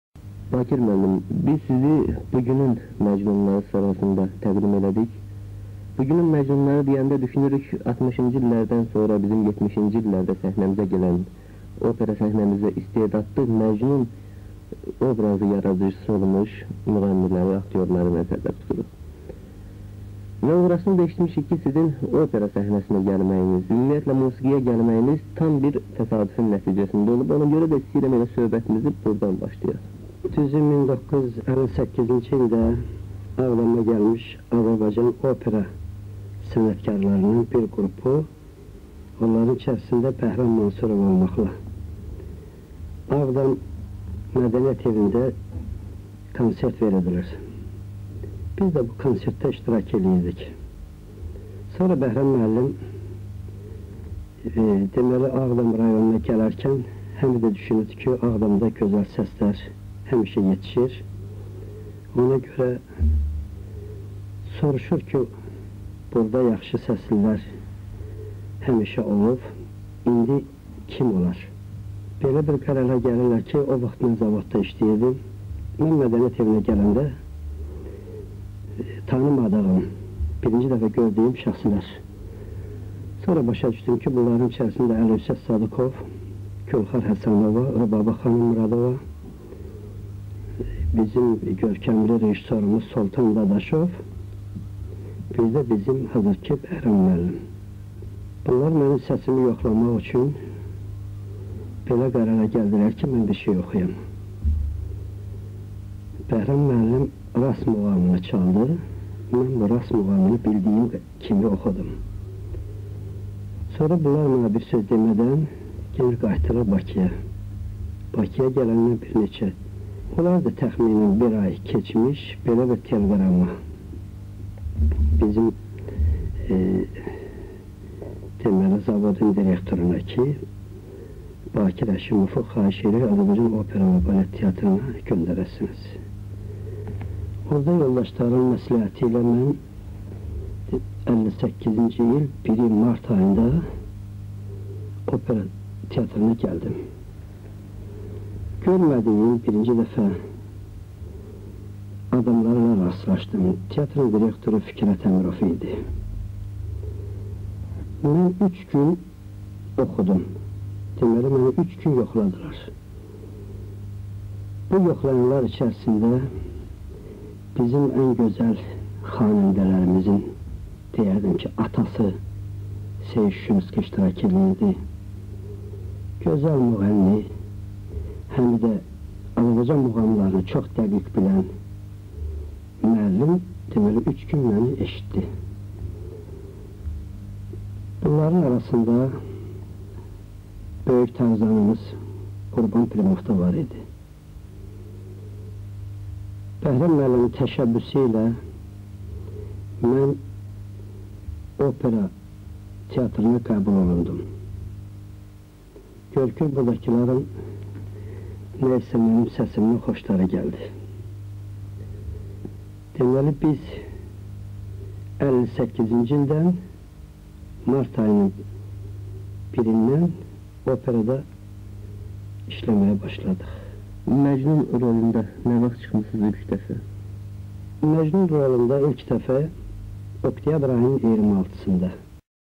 Bakir Haşımovun çıxışı | AzRadio | 18.11.1978
Xanəndə Bakir Haşımovun Azərbaycan radiosunda çıxışı (18 noyabr 1978-ci il).